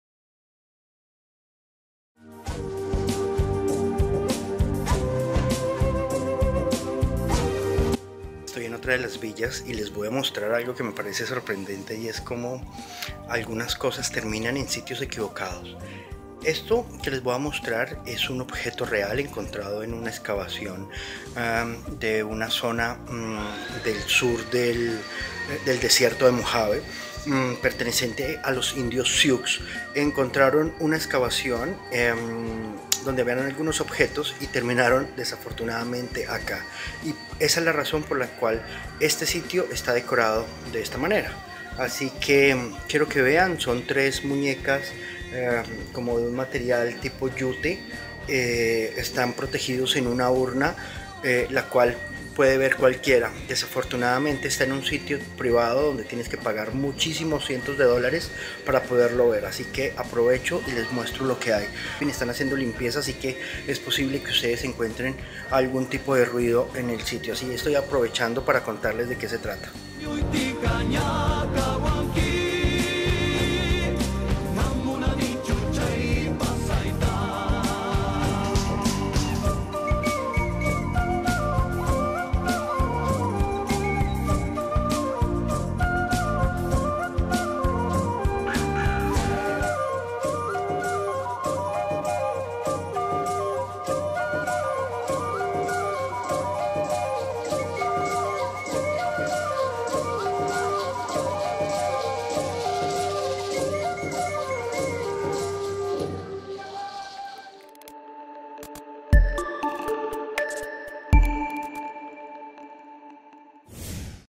Un pequeño momento de mi día . (Estoy en el lugar equivocado)
Algunos objetos en este sitio parecen estar en el lugar equivocado.
La ambición de algunos desvanece la identidad de un pueblo que pide a gritos ser escuchado.